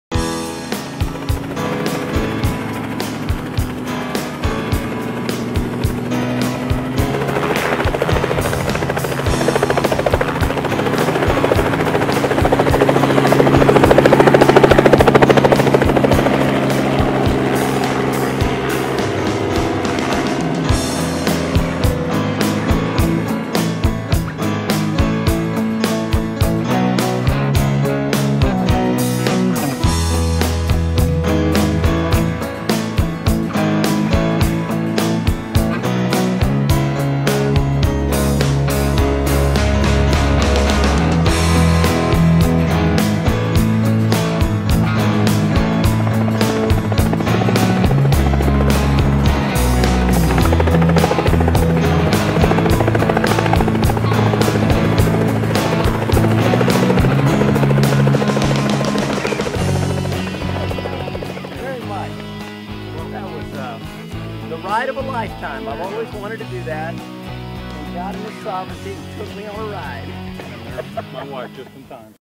TERRY DILLON CHOPPER JUNE 2010
My neighbor Terry lands his helicopter in a lot next to his house!  He took a new subdivision resident up for a ride.